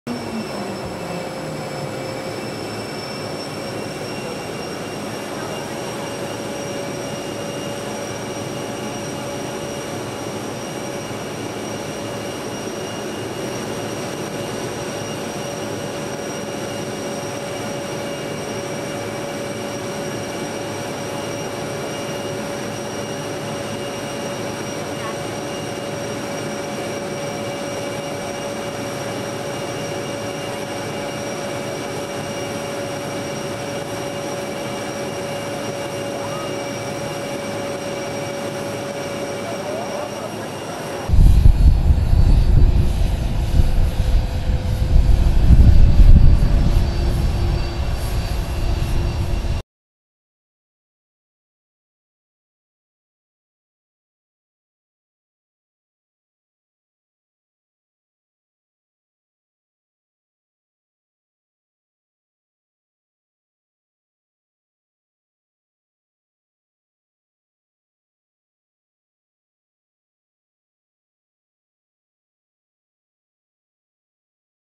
US President Joe Biden heads to India for G20 meeting
India is all set to host the G20 Summit in New Delhi from September 9-10. The summit will be hosted at the state-of-the-art Bharat Mandapam Convention Centre at Pragati Maidan, New Delhi